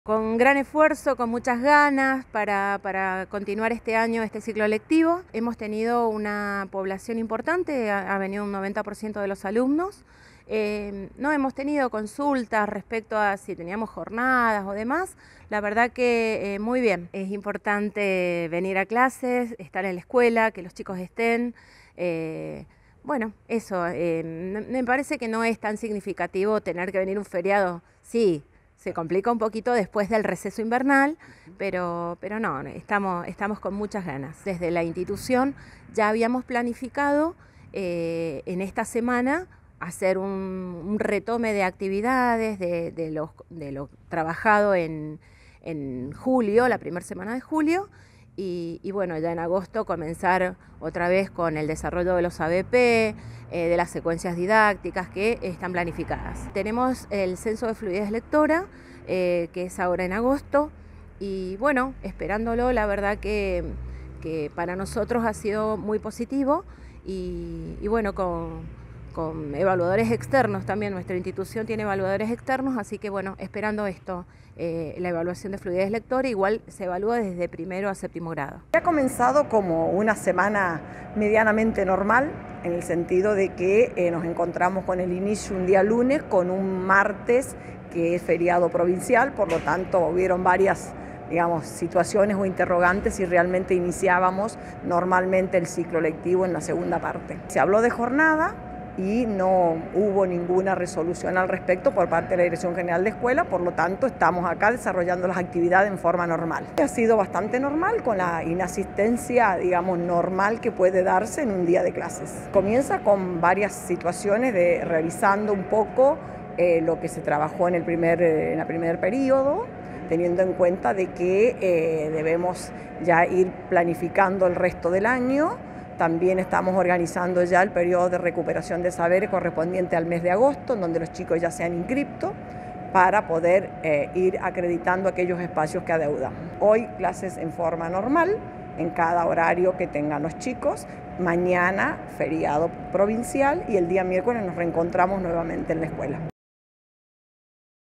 NORMAL COMIENZO DE CLASES
A pesar de las especulaciones por el feriado del martes la concurrencia de alumnos en las escuelas del departamento fue casi normal, así quedó demostrado en una recorrida que hizo Noticiero Andino por algunas de las escuelas cabeceras tanto a nivel primario como secundario del departamento.